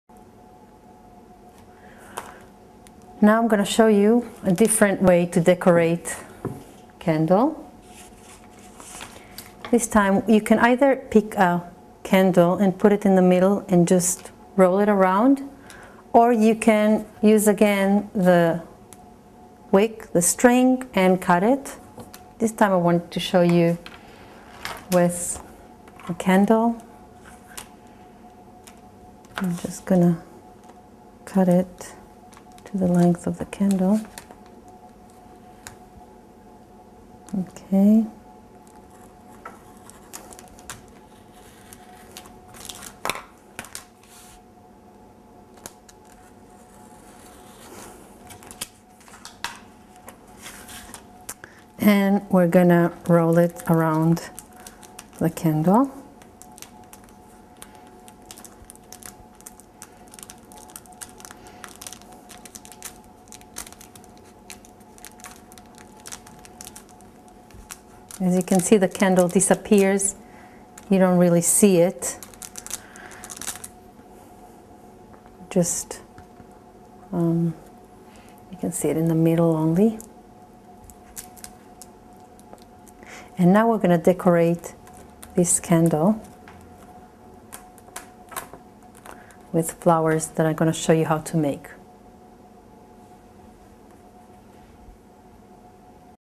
How to Add a Wax Layer to a Homemade Candle
How to Add a Wax Layer to a Homemade Candle. Part of the series: How to Make Decorative Candles. Learn how to add a wax layer to make your own homemade candle in this arts and  crafts video on candle making.